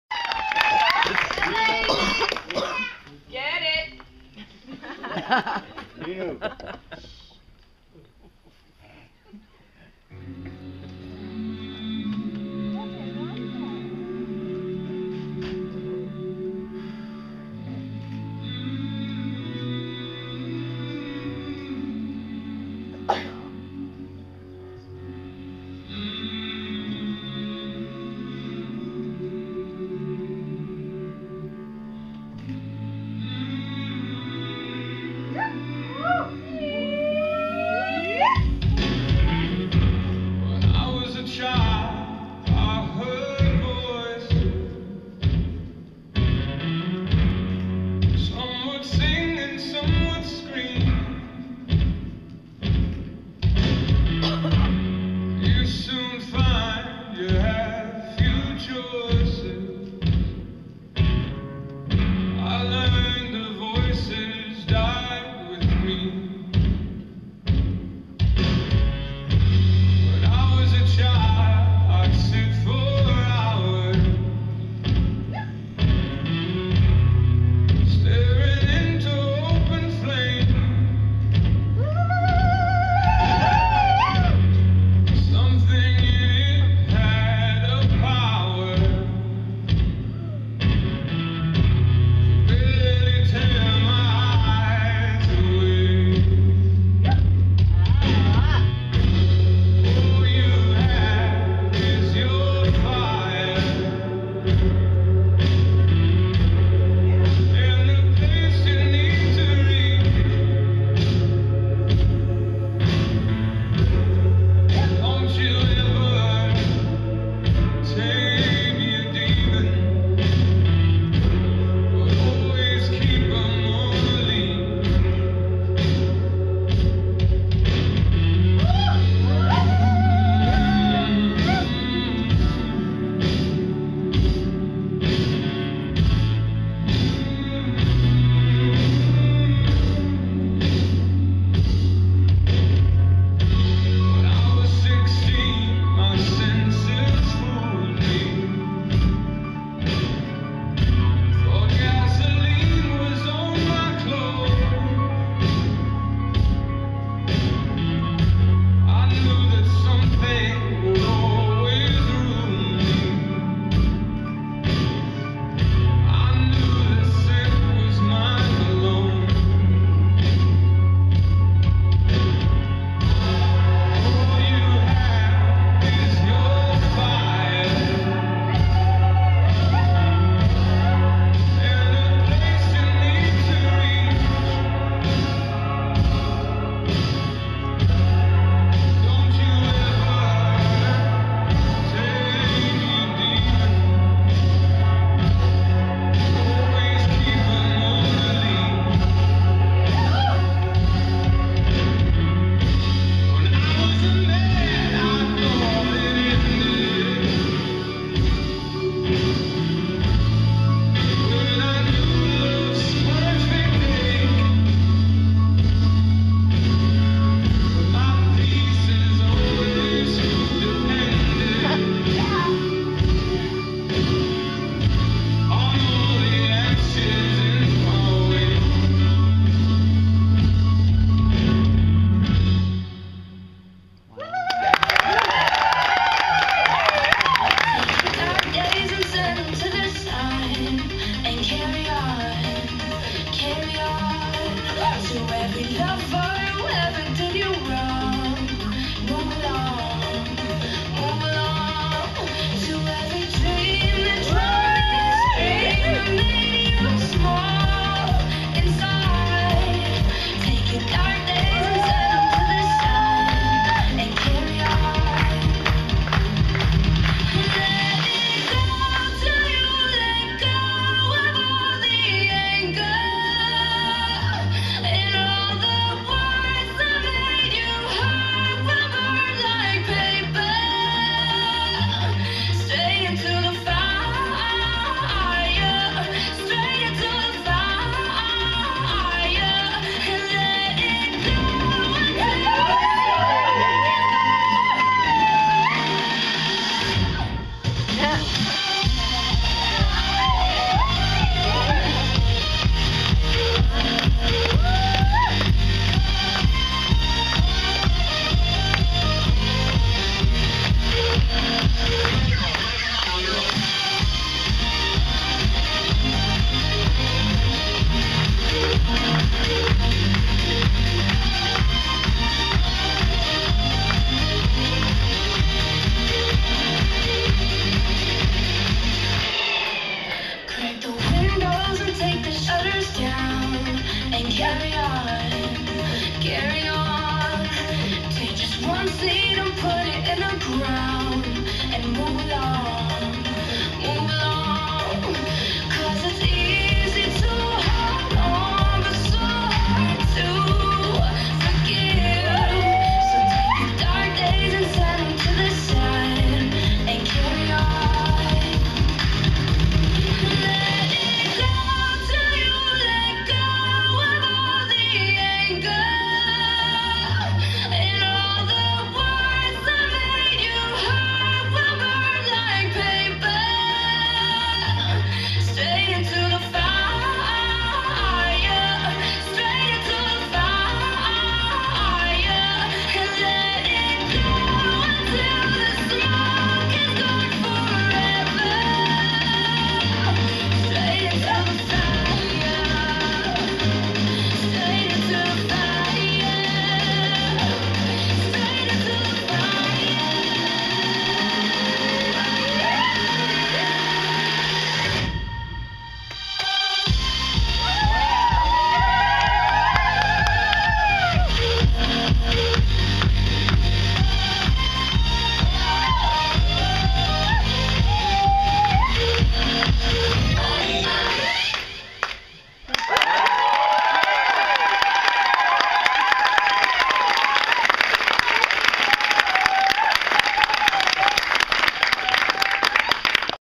Raqdoll at Hipnosis Spring Gala 2016
The lovely ladies of Raqdoll bring you their "Fire Set" performance at the Hipnosis Spring Gala 2016 on Saturday, April 23rd, 2016.  We hope that you enjoy our performance as much as we did sharing it with you!      Many thanks to the ladies of Hipnosis and the Tribal Bellies family for having us!